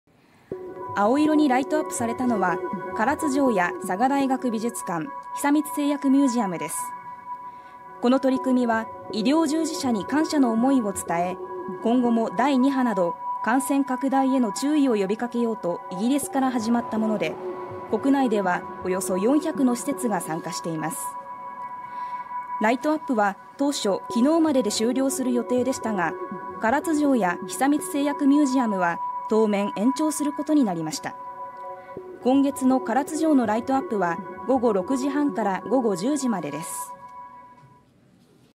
唐津城も青色に 医療従事者への感謝を込め全国で一斉にブルーライトアップ【佐賀県】 (20/06/01 12:03)
医療従事者への感謝を伝え、今後の感染拡大への注意喚起をしようと31日夜、日本各地で一斉にライトアップが行われ、県内の施設でも建物が青色に照らされました。

青色にライトアップされたのは唐津城や佐賀大学美術館、久光製薬ミュージアムです。この取り組みは医療従事者に感謝の思いを伝え、今後も第二波など、感染拡大への注意を呼びかけようとイギリスから始まったもので国内では約400の施設が参加しています。ライトアップは当初31日までで終了する予定でしたが、唐津城や久光製薬ミュージアムは事態が終息するまで延長することになりました。

6月の唐津城のライトアップは午後6時半から午後10時までです。